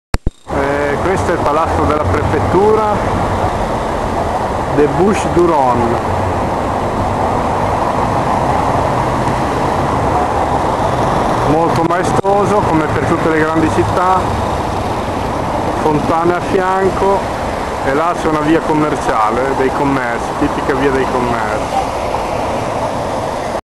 Video di viaggio 053 (journey clip 053)
Palazzo della Prefettura di Marsiglia. Qui sono nel pieno centro della città.